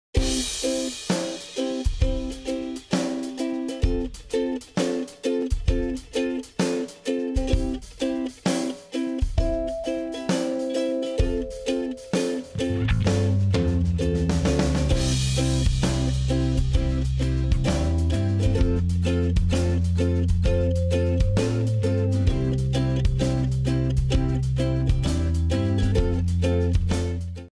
Transforming pictures into beautiful mosaics | Scott the Engineer
I used a processing sketck to turn pictures into a stain glass mosaic.